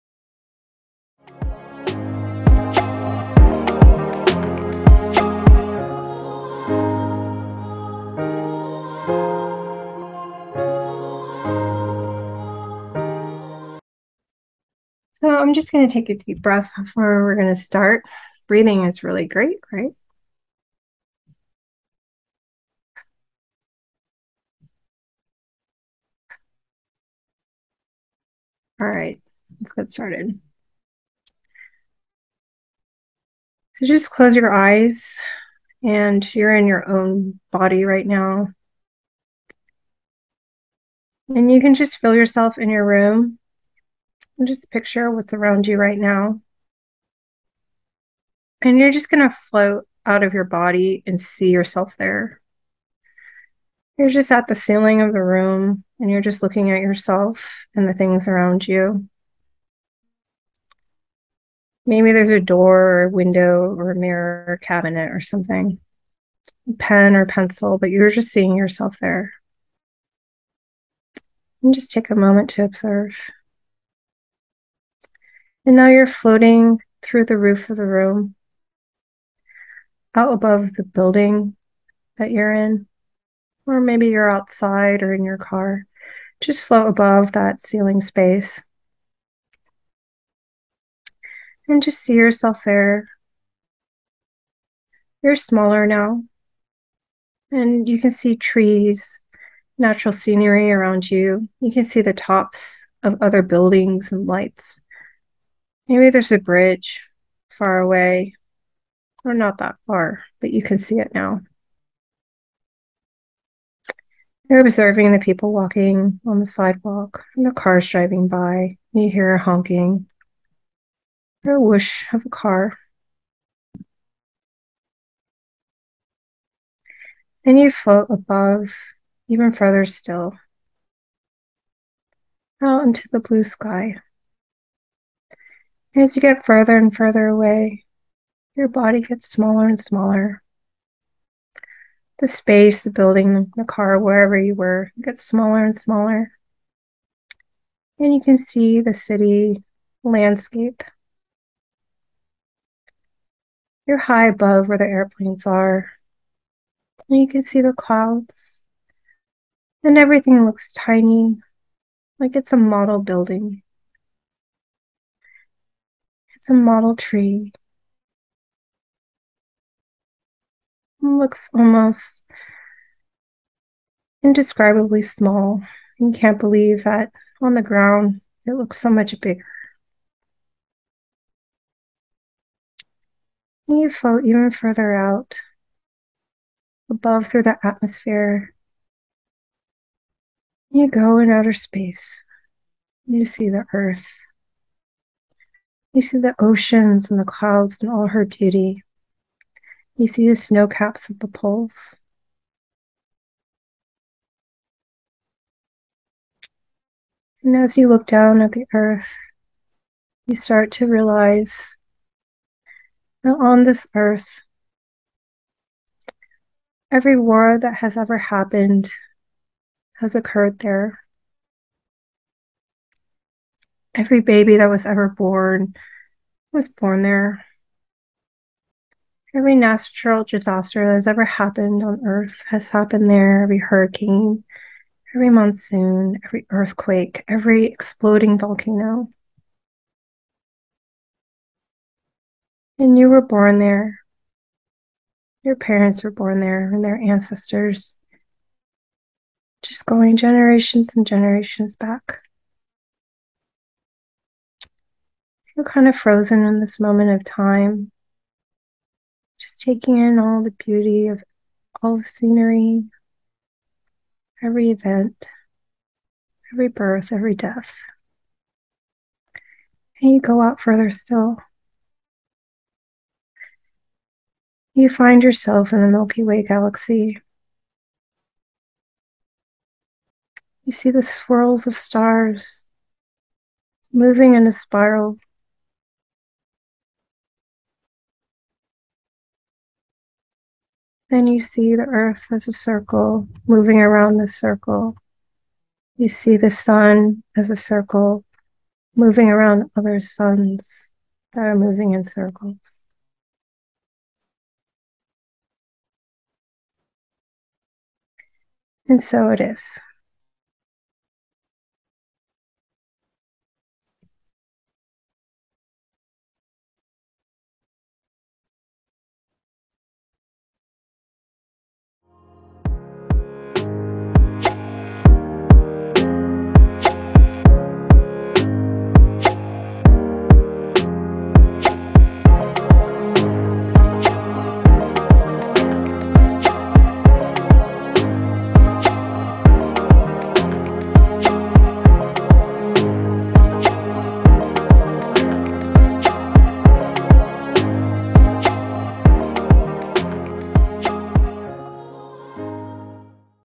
6-Minute Anger Detox Meditation
This is a quick way to get your anger under control.  Take the 6 minutes to gain perspective in those moments of anger.

Visit Nelly at ModernShamanExperience.com for a healing session